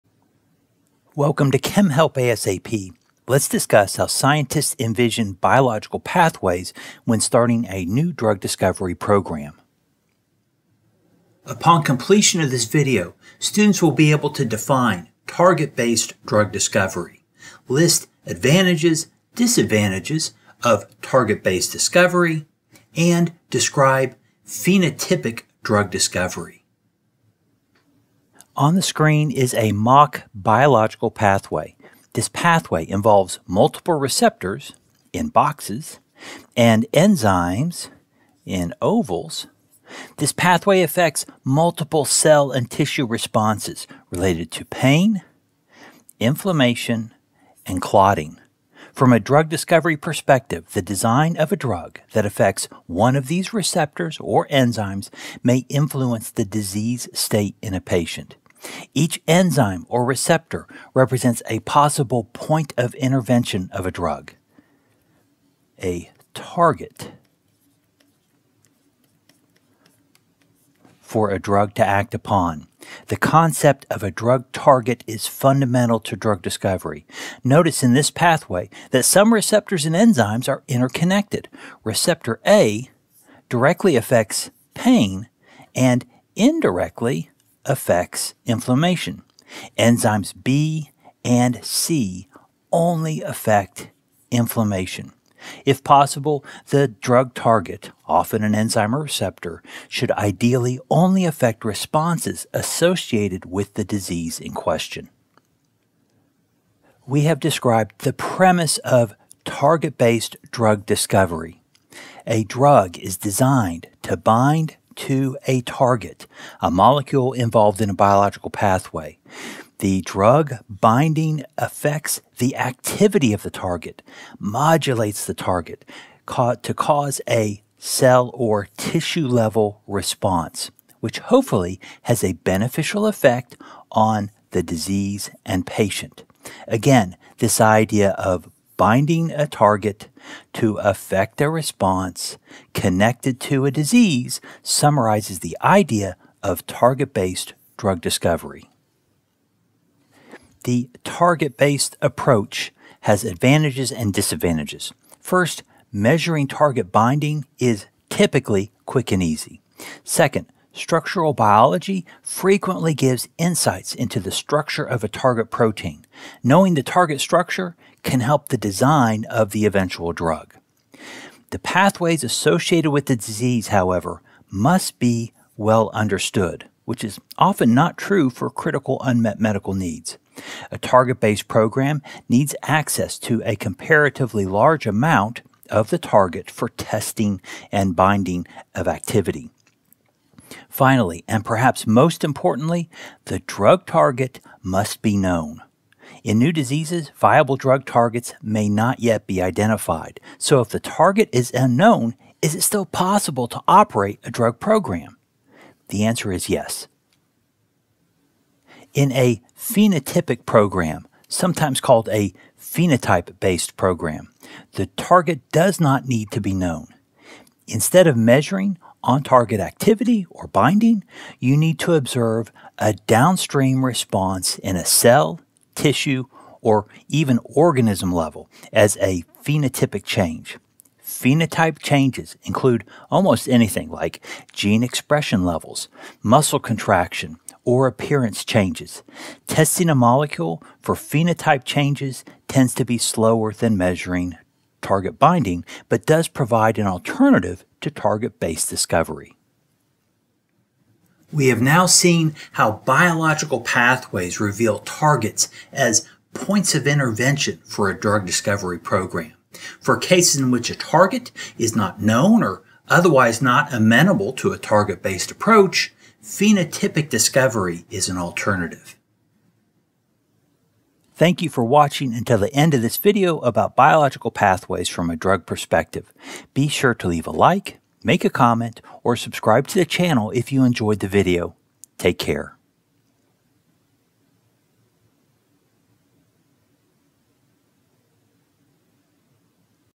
pathways, targets, & responses in drug discovery
Biological pathways involves multiple receptors (in boxes) and enzymes (in ovals).  Pathways can affect multiple cell and tissue responses.  From a drug discovery perspective, the design of a drug that affects one of receptor or enzyme to influence the disease state in a patient.  Each enzyme or receptor represents a possible point of intervention of a drug, a target for a drug to act upon.  The concept of a drug “target” is fundamental to drug discovery.  If possible, the drug target (often an enzyme or receptor) should ideally only affect responses associated with the disease in question.
This process describes the premise of target-based drug discovery.  A drug is designed to bind to a target – a molecule involved in a biological pathway.  The drug binding affects the activity of the target – modulates the target – to cause a cellular or tissue-level response, which hopefully has a beneficial effect on the disease and patient.  Again, this idea of binding a target to affect a response connected to the disease summarizes the idea of target-based drug discovery.
The target-based approach has advantages and disadvantages.  First, measuring target binding is typically quick and easy.  Second, structural biology frequently gives insight into the structure of a target protein.  Knowing the target structure can help the design of the eventual drug.  The pathways associated with the disease, however, must be well understood, which is often not true for critical unmet medical needs.  A target-based program needs access to a comparatively large amount of the target for testing and binding of activity.  Finally, and perhaps most importantly, the drug target must be known.  In new diseases, viable drug targets may not yet be identified.  So, if the target is unknown, is it still possible to operate a drug program?  The answer is yes.
In a phenotypic program, sometimes called a phenotype-based program, the target does not need to be known.  Instead of measuring on-target activity or binding, you’d need to observe a downstream response in a cell, tissue, or even organism level as a phenotypic change.  Phenotype changes include almost anything, like gene expression levels, muscle contraction, or appearance changes.  Testing a molecule for phenotype changes tends to be slower than measuring target binding but does provide an alternative to target-based discovery.